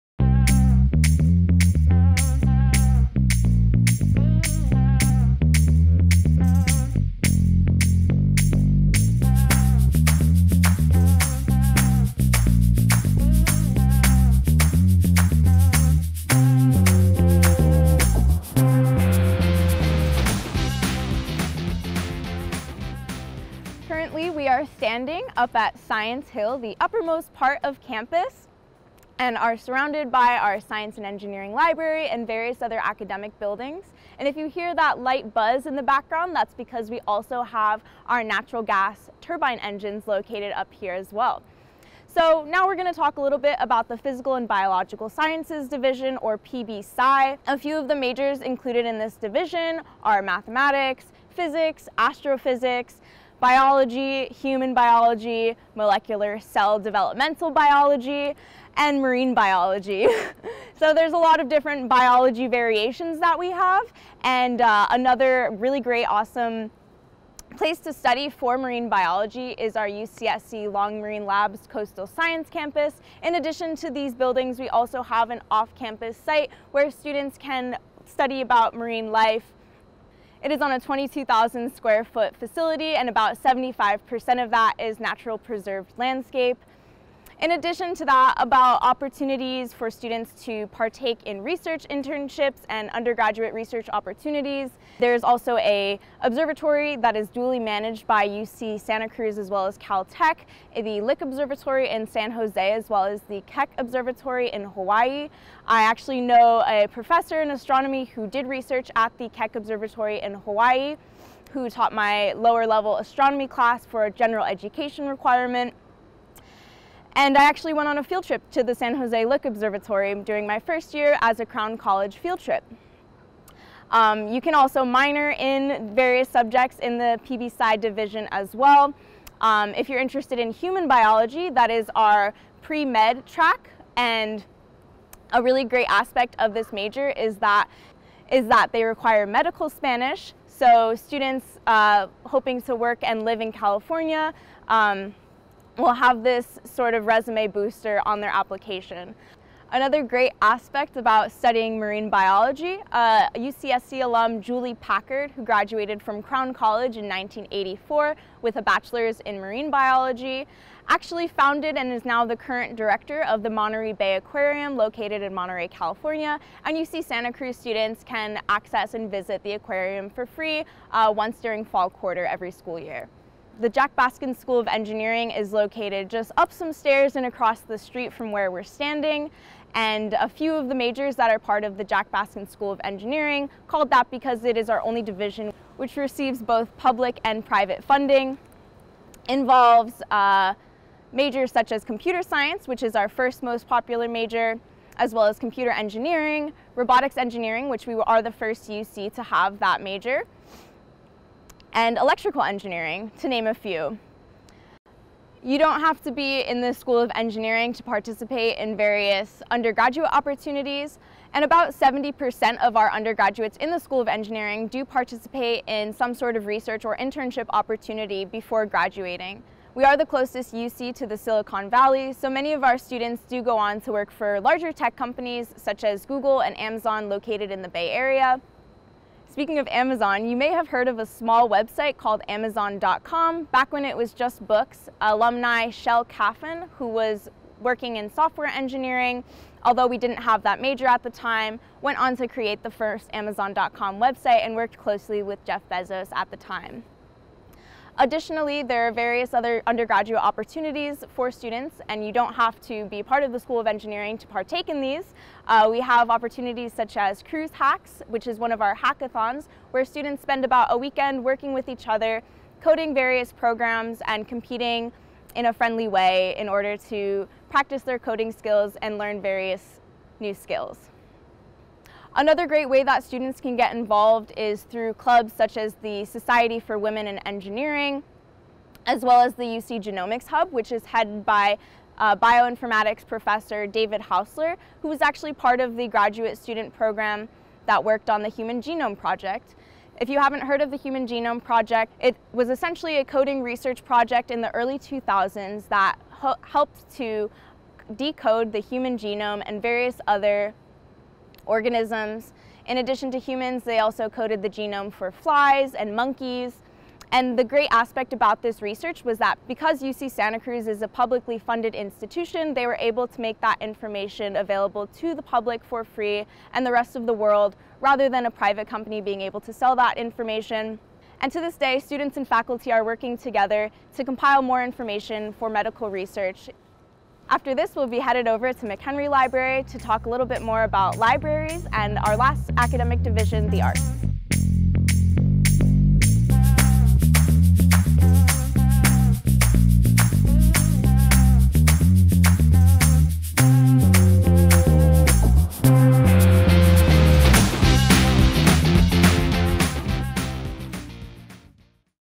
UCSC Campus Tour Ch 10: Physical & Biological Science Division and Jack Baskin School of Engineering
You are entering Banana Slug Territory! Tour the campus between the mountains and the sea. Led by our Student Life & University Guides (S.L.U.G.s), explore what it means to learn, live, and grow in our community.

This is the 10th video in a 13 chapter series. Head to Science Hill to learn more about UCSC's Physical & Biological Sciences Division and the Jack Baskin School of Engineering (starting at the 2:53 timestamp) with an overview of facilities, programs of study, some notable discoveries and alumni.